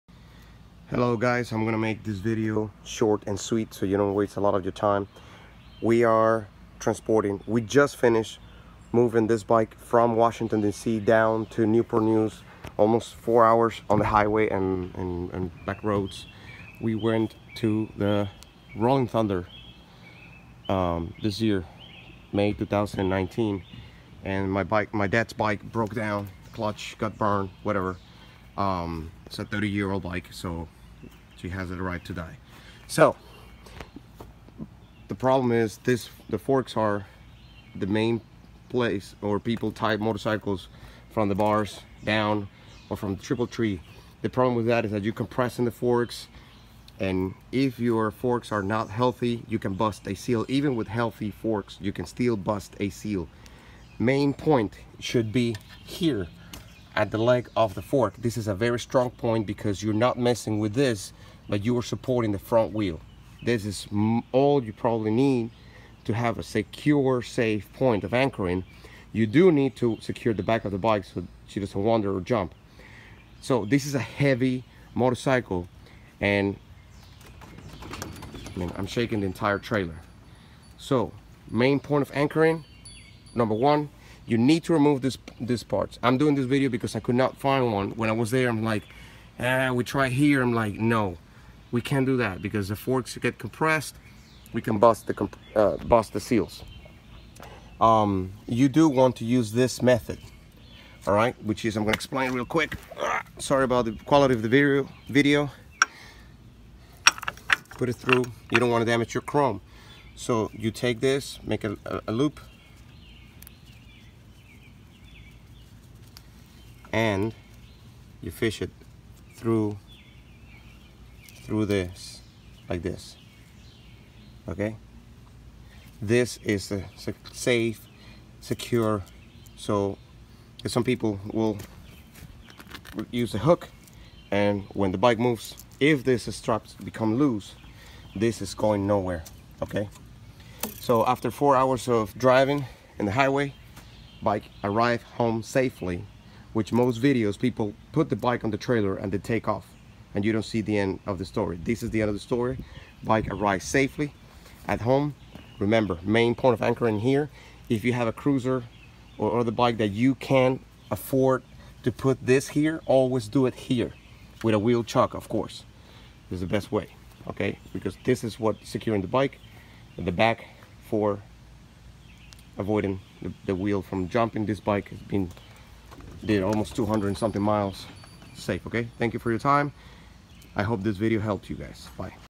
The CORRECT way to transport, haul, tow a Goldwing 1500 or any big motorcycle
This is the right way to transport a big bike without damaging anything while being safe.
For this specific bike you must remove the plastic cover that hide the disco brakes.